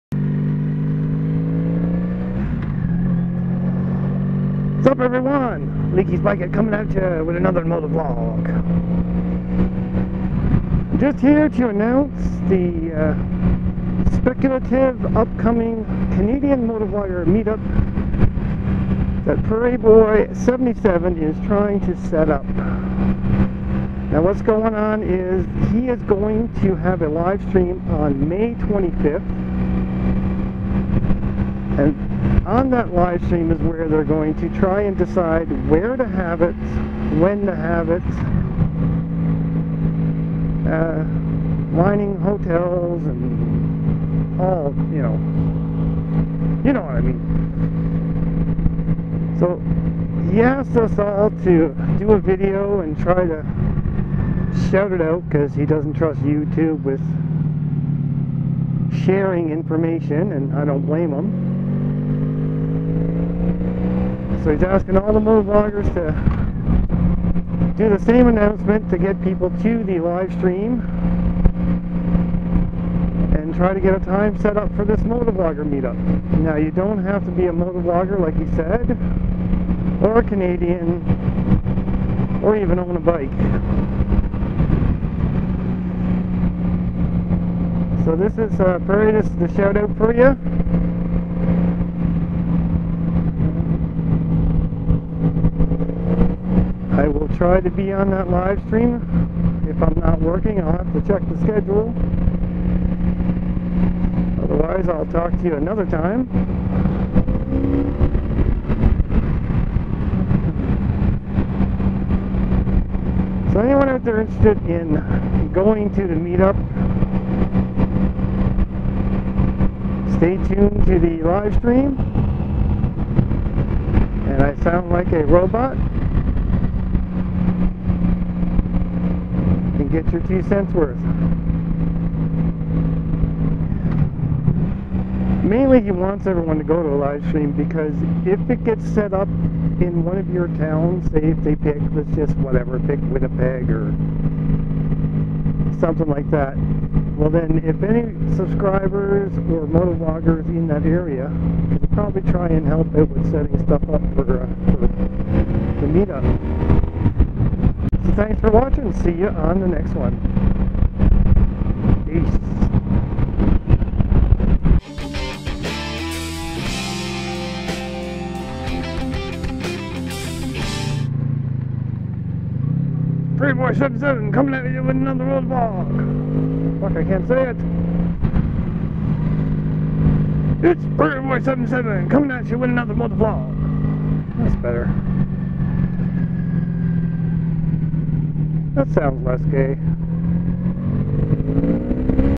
Canadian Motovlogger Meet-up / Prairie Boy77
Spreading the word to try and get a Canadian Motovlogger meet up.  Go to Prairie Boy77 Channel on May 25th to find out more.  Link to his channel Below

Prairie Boy77